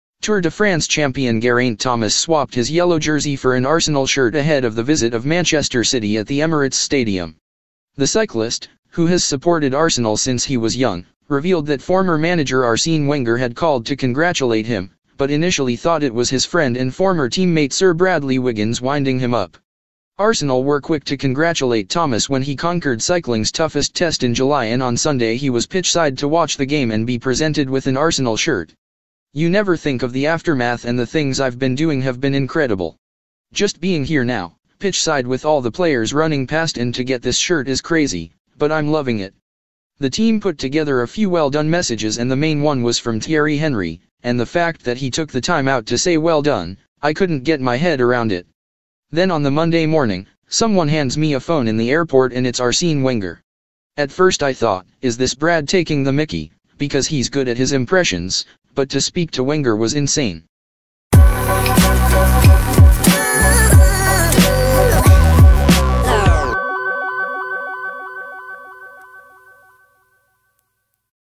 Geraint Thomas swaps yellow jersey for red at Arsenal
Tour de France champion Geraint Thomas swapped his yellow jersey for an Arsenal shirt ahead of the visit of Manchester City at the Emirates Stadium.